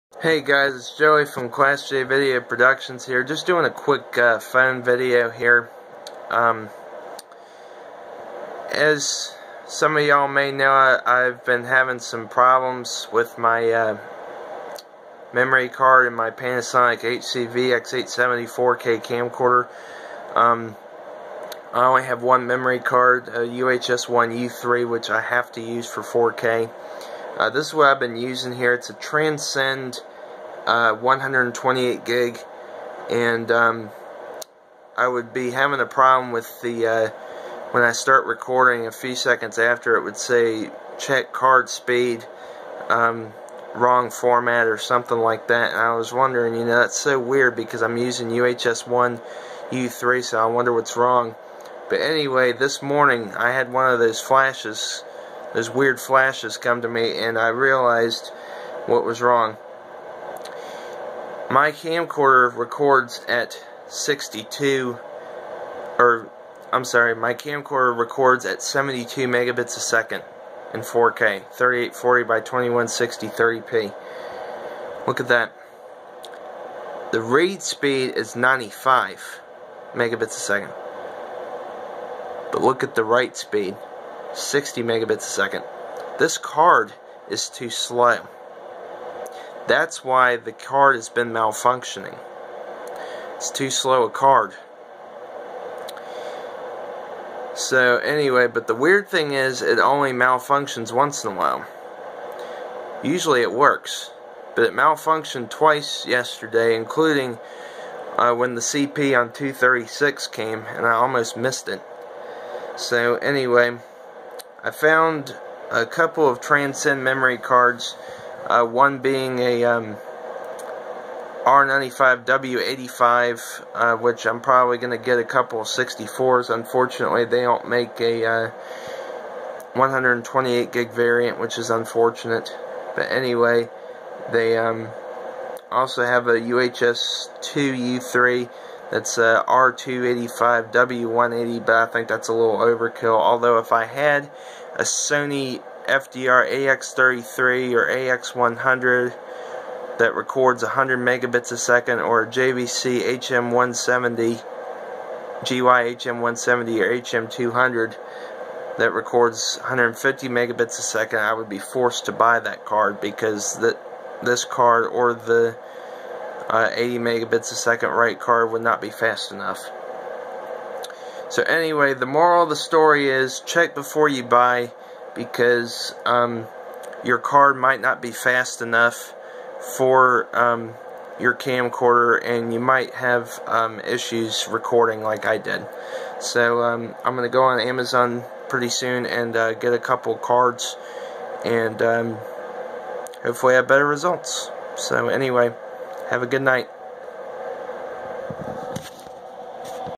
Talking about ongoing memory card issues
I have been having problems with my memory card on my Panasonic HC-VX870 camcorder. Every once in a while, it will say "Stopping recording. Check write speed." I thought it was just a bad card. Upon further examination, I found it's too slow a card. The write speed is 60mbps, the camcorder records at 72mbps. I will be getting a faster card very soon. For those of you with a 4K camcorder, check your video bit rate and the card speed before you buy a card so you don't have the issues that I have had. Shot in 4K with a Motorola Droid Turbo 2.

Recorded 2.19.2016